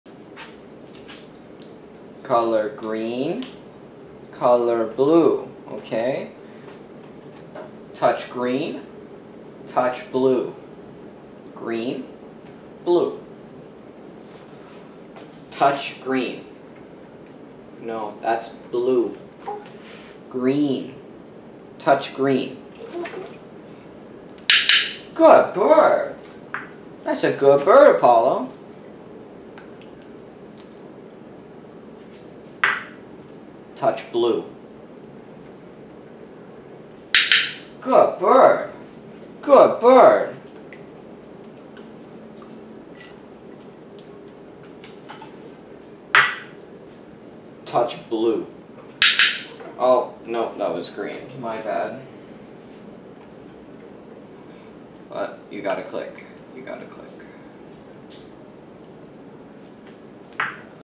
The First Recorded Training Session
This is the first time we ever recorded training Apollo, we lost all of the date/time data off of the camcorder footage, but we estimate this to be in the first half of 2021 when he was about 1 1/2 years old.